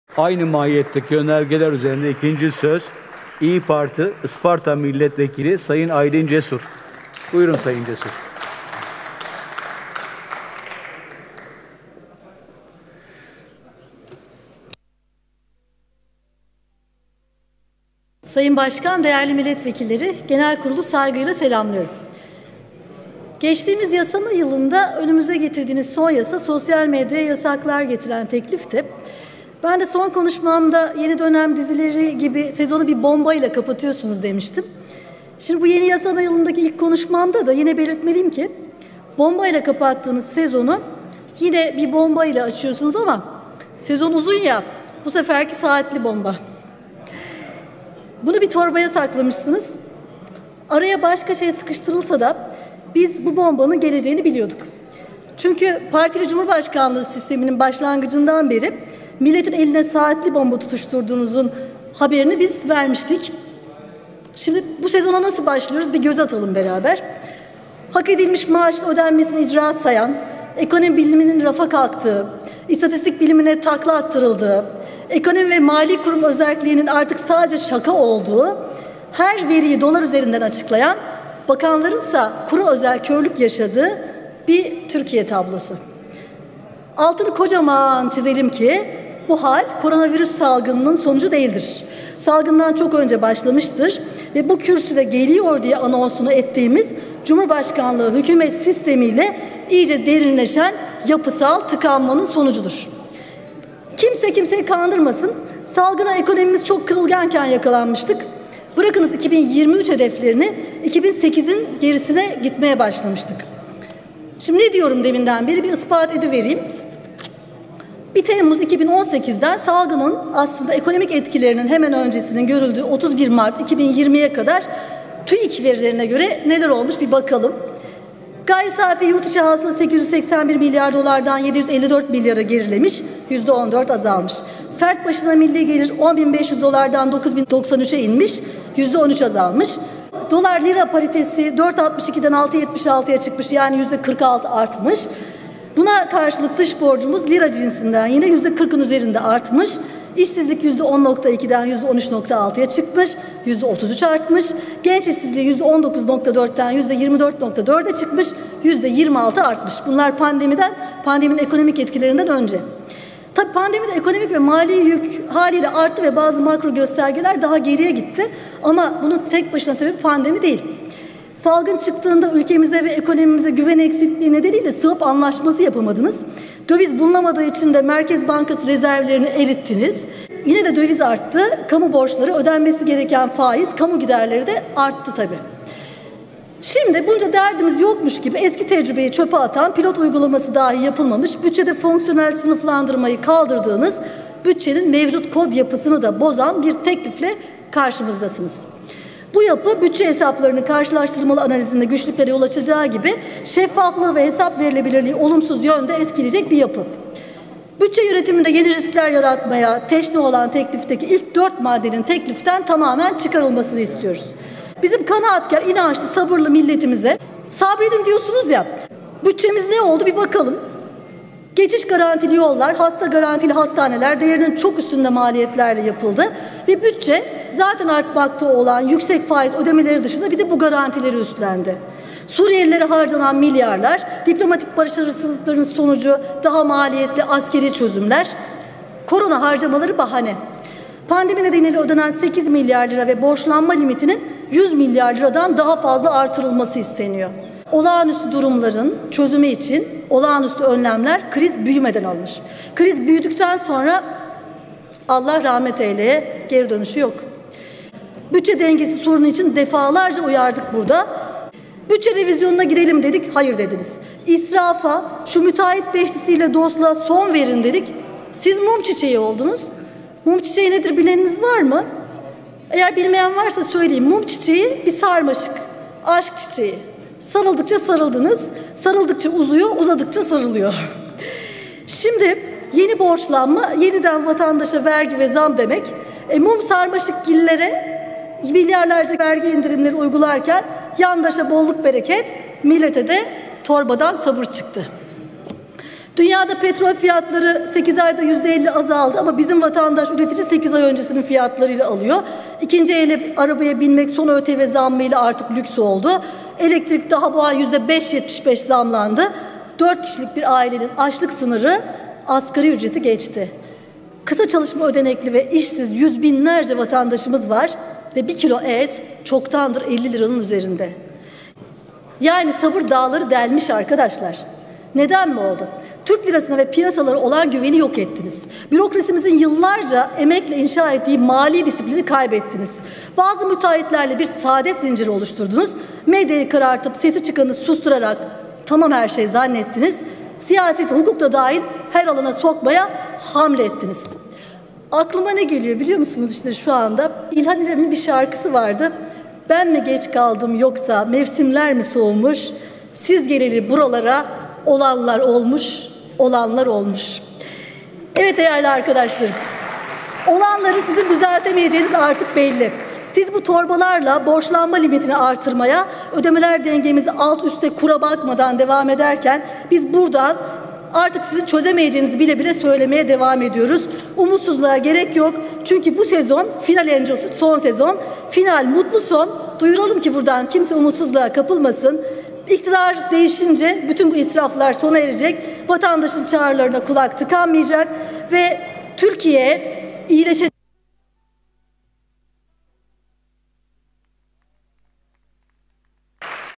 İYİ Parti Isparta Milletvekili Dr. Aylin Cesur - 2020 Ekiminde Türkiye'nin Durumu
İYİ Parti Isparta Milletvekili Dr. Aylin Cesur - Kamu Mali Yönetimi ve Kontrol Kanunu İle Bazı Kanunlarda Değişiklik Yapılması Hakkında Kanun Teklifi Üzerine 4. Madde TBMM Genel Kurul Konuşması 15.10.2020